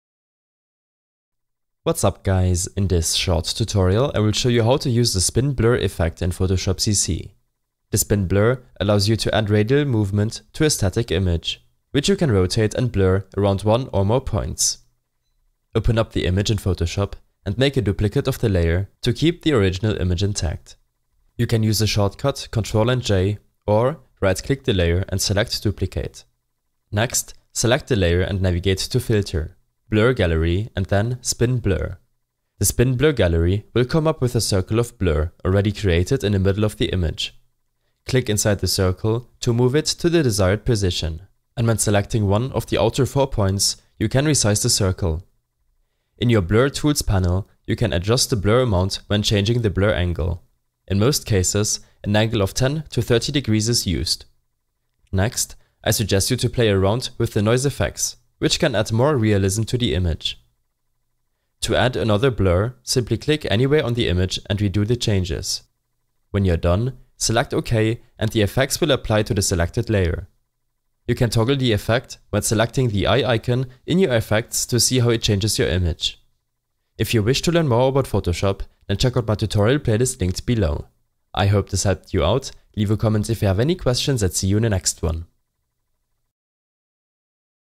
Photoshop CC - How to Use the Spin Blur Effect
A short tutorial on how to use the spin blur effect in Photoshop CC.